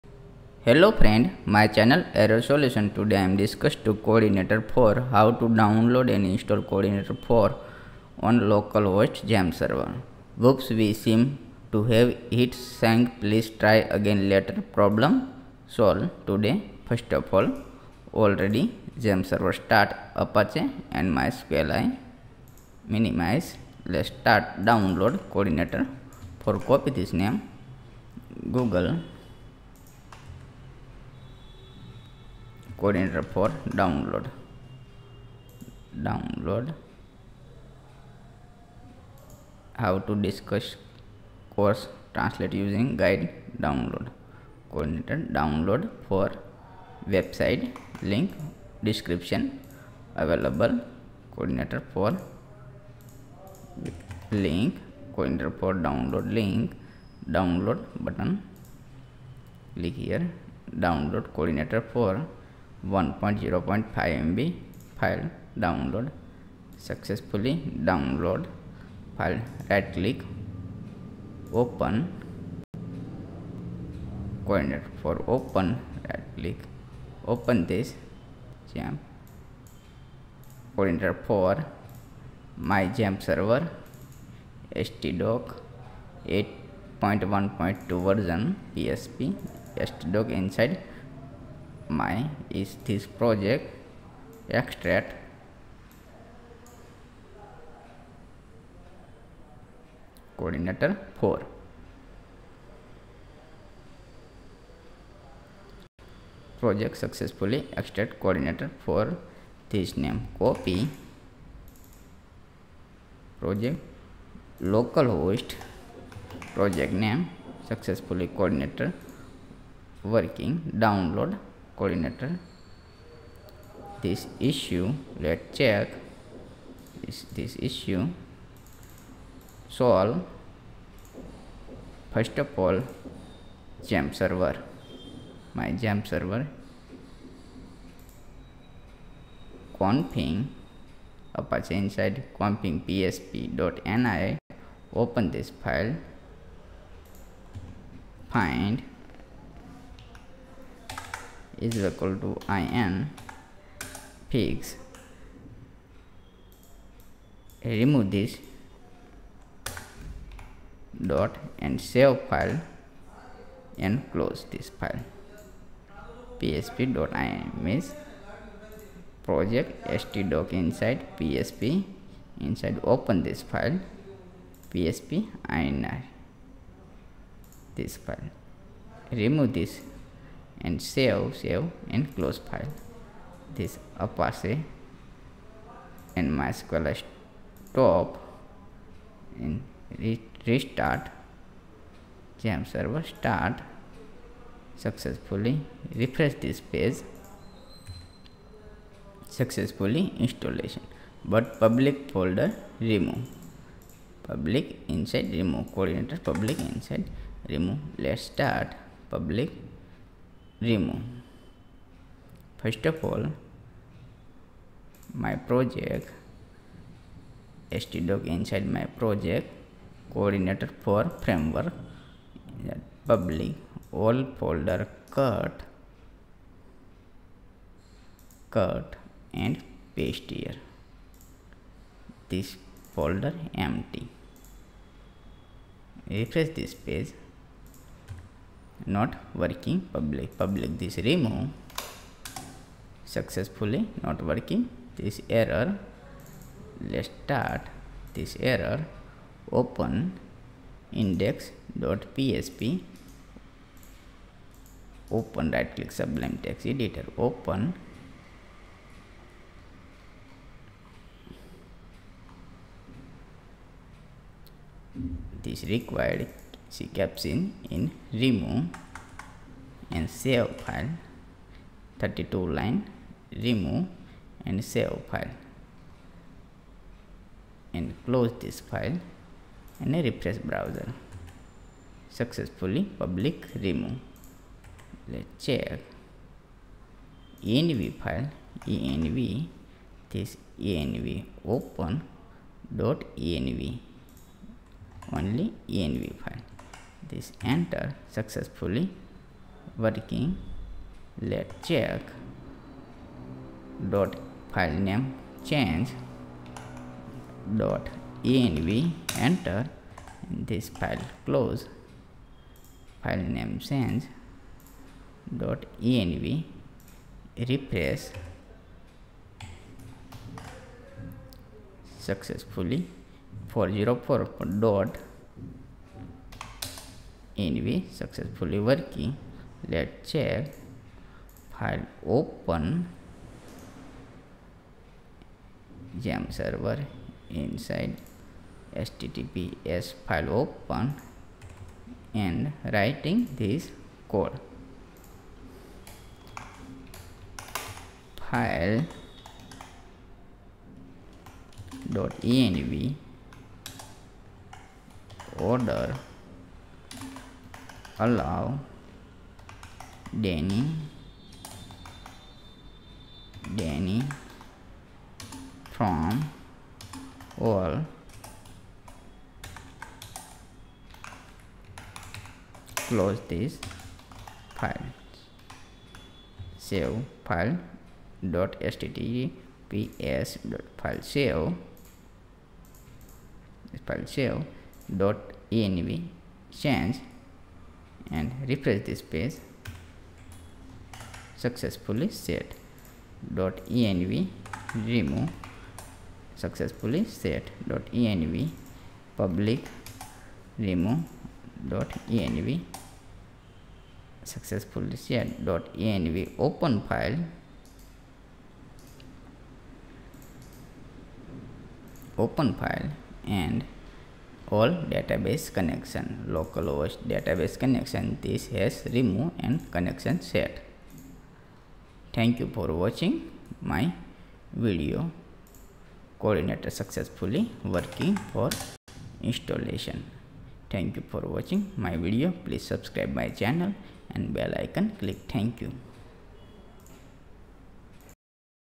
How to Download and Install Codeigniter 4 on Localhost Xampp | Whoops! We seem to have... Problems
How to Download and Install Codeigniter 4 on Localhost Xampp | Whoops! We seem to have hit a sang. Please try again later.. | Problems

File Open :- php.ini
Find :- ;extension=intl
Remove:- ;
Add :- extension=intl

File Open :- .htaccess
This code add :- Video show

File name Change :- env to .env